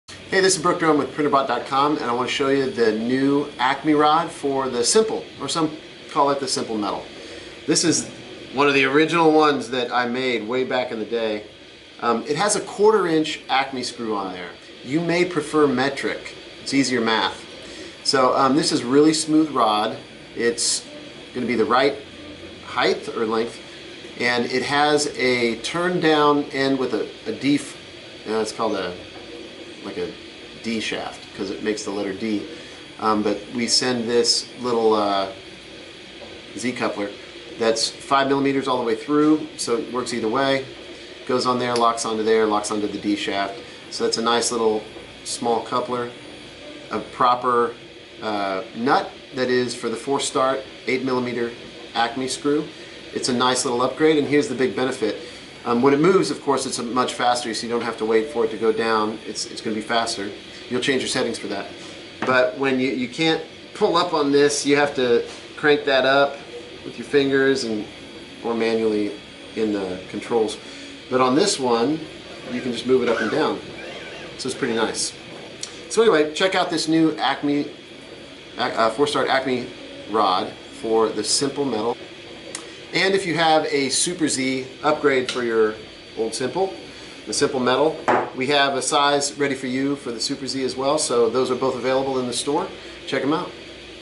Printrbot Simple 4 start 8mm Acme rod upgrade
Upgrade your Printrbot Simple with a metric 4-start Acme rod. The settings are easy and even math :) It moves the Z axis way faster and replaces the english 1/4" acme it was shipped with originally. You do have to change the steps per inch with a quick command in your host software. Enjoy!